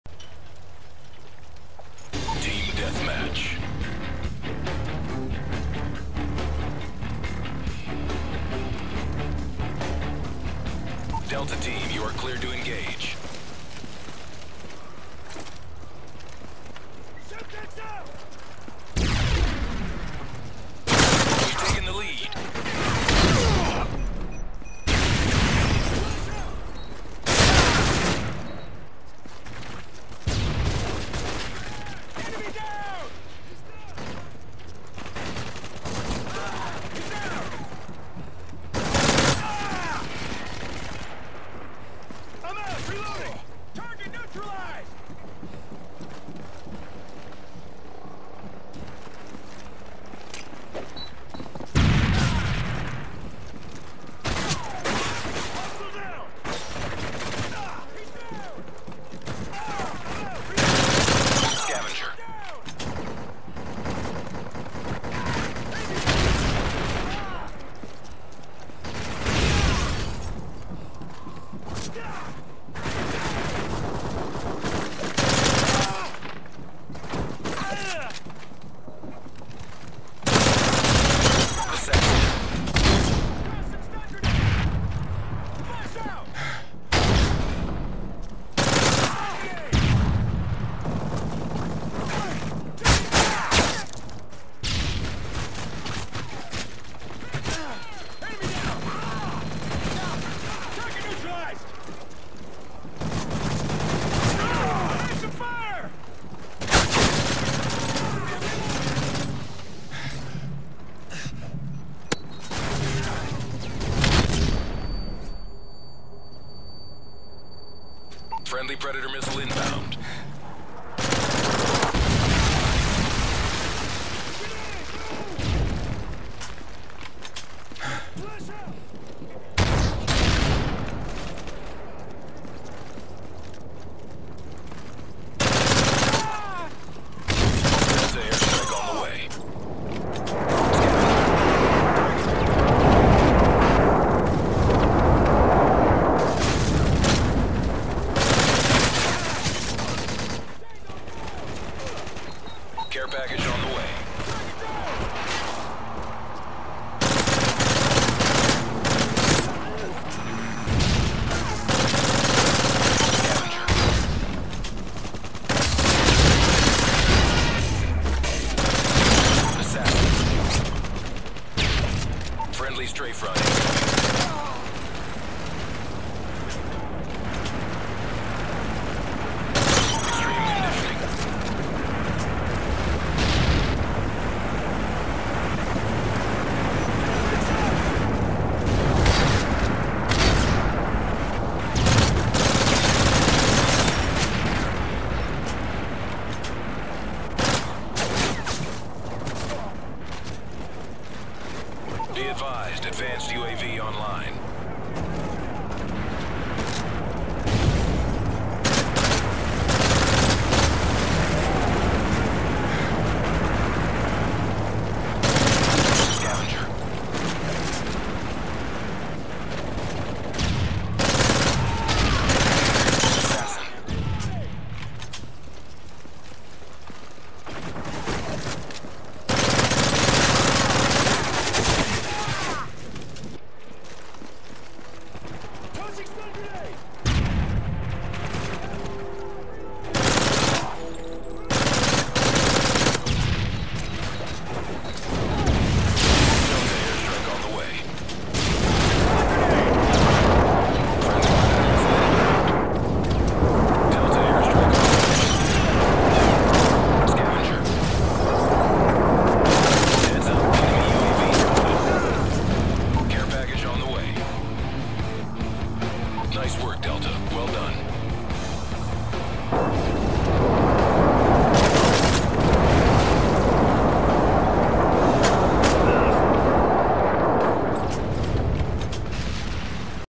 Pretty good game
This was the game after the last seatown game I was bent on getting you guys 5 videos in a matter of 4 hours. Hopefully I can upload 4-5 vids a day as I am a nerd.